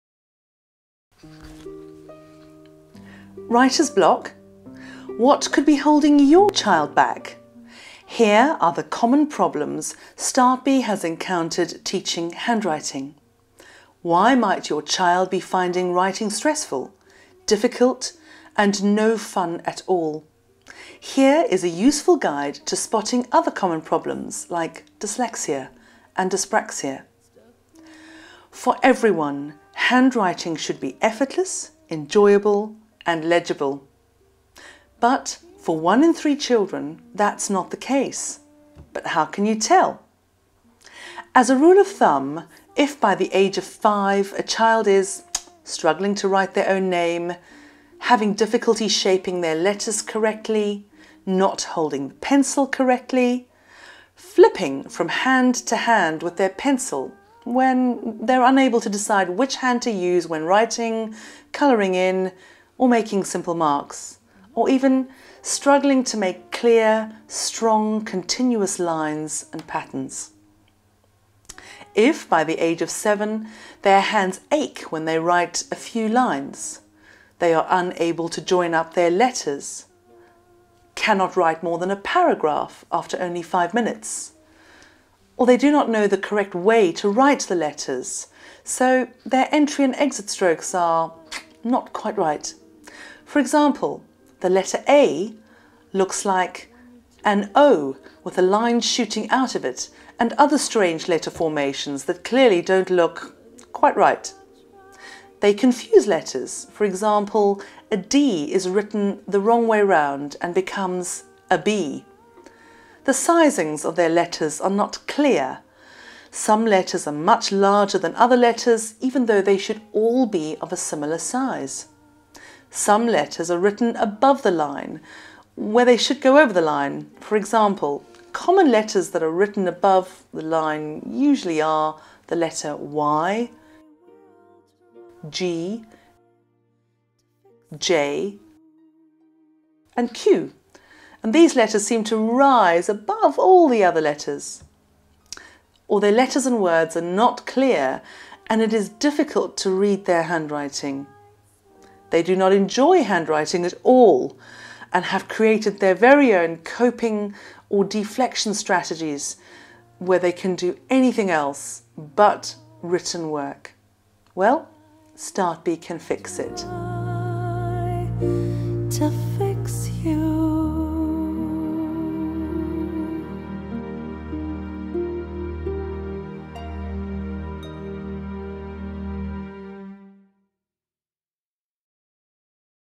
Handwriting Anomalies (Issues to look out for if your child is struggling to write)
Handwriting Anomalies and signs to look out for if your child is struggling with handwriting, making marks, holding the pencil incorrectly/correctly, right hand/left hand flipping, colouring in, correct pencil grip, correct entry and exit strokes for letters of the alphabet, creating their letters so that they can join up (cursive writing), letter shapes, letter sizes, where letters "sit" on the grid lines and "attitudes" towards handwriting in school and at play.